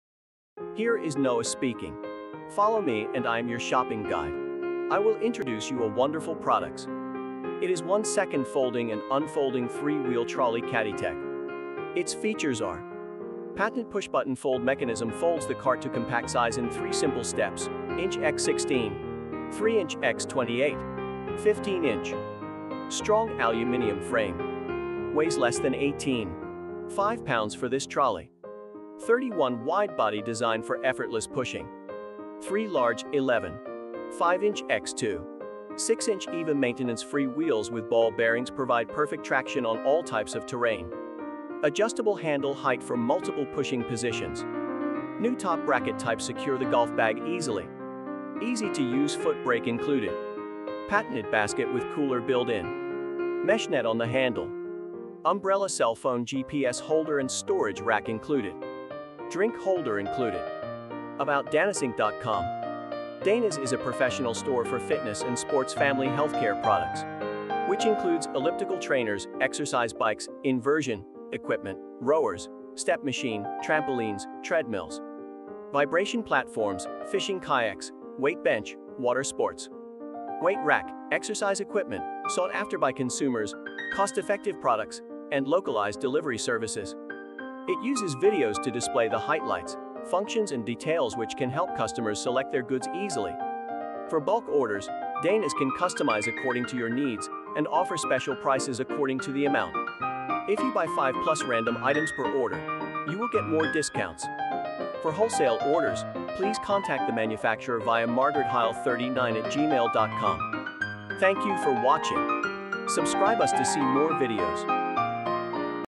𝐖𝐡𝐞𝐞𝐥 𝐩𝐮𝐬𝐡 𝐜𝐚𝐫𝐭 | 𝐄𝐥𝐚𝐬𝐭𝐢𝐜 𝐬𝐭𝐫𝐚𝐩 | 𝐋𝐢𝐠𝐡𝐭 𝐰𝐞𝐢𝐠𝐡𝐭 | 𝐀𝐥𝐮𝐦𝐢𝐧𝐮𝐦 𝐟𝐫𝐚𝐦𝐞 𝐜𝐨𝐧𝐬𝐭𝐫𝐮𝐜𝐭𝐢𝐨𝐧 | 𝐖𝐢𝐝𝐞 𝐛𝐨𝐝𝐲 𝐝𝐞𝐬𝐢𝐠𝐧.
The golf push carts 3 wheel folding golf hand cart is made w/ heavy-duty aluminum frame construction material & equipped w/ upper/lower brackets w/ elastic strap & patented bag holder. Features compact size for easy storage & transport.
This golf pull carts collapsible golf bag push cart features ergonomic padded height adjustable handle for multiple pushing positions and to accommodate users of different heights.
The lightweight push cart walk golf cart features foot brake for rear wheel & handle brake to control 360° swivel for front wheel.